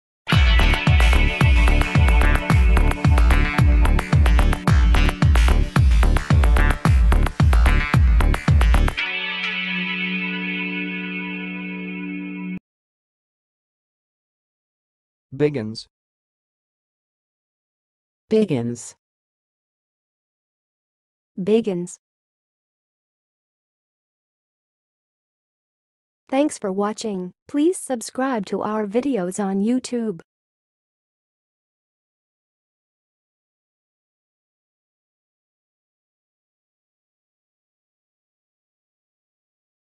How to say "biggins"! (High Quality Voices)
Watch in this video how to say and pronounce "biggins"!